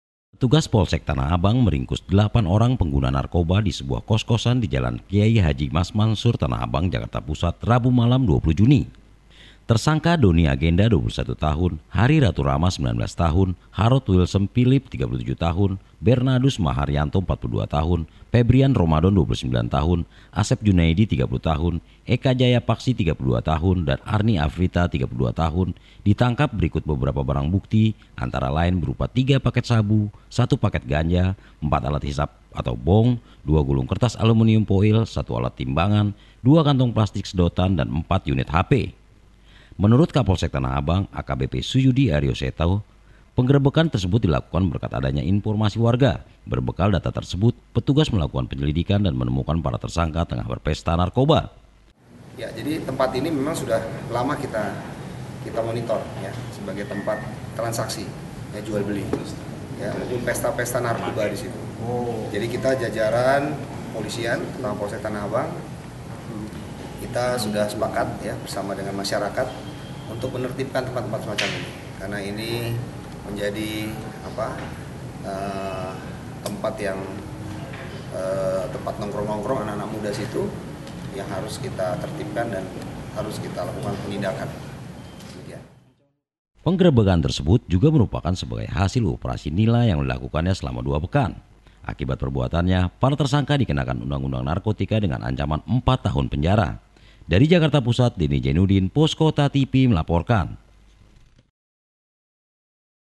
Polsek Tanah Abang Tangkap 8 Pengguna Narkoba
TANAH ABANG (Pos Kota) -- Petugas Polsek Tanah Abang meringkus delapan orang pengguna narkoba di Jalan KH Mas Mansyur, Tanah Abang, Jakarta Pusat, Rabu (20/6) malam.